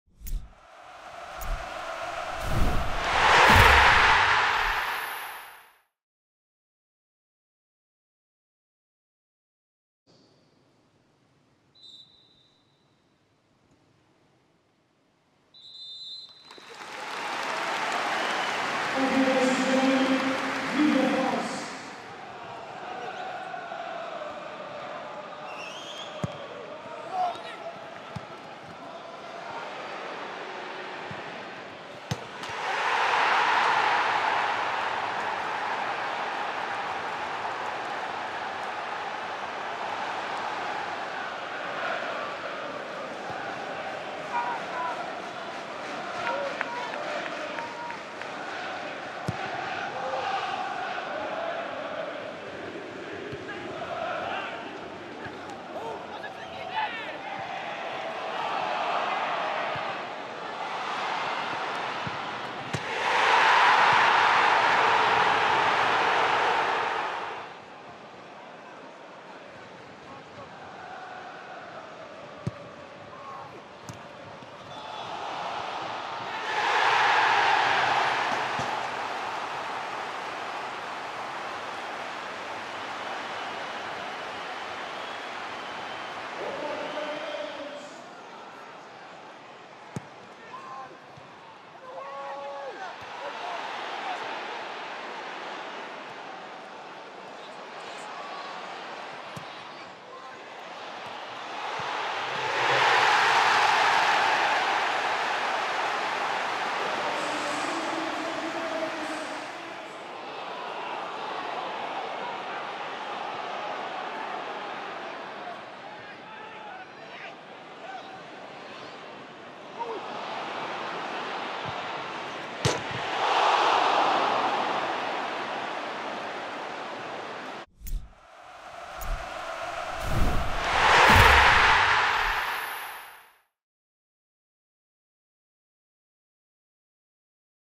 Blades 2-2 Southend - match action
Brief highlights from the Bramall Lane draw with Southend United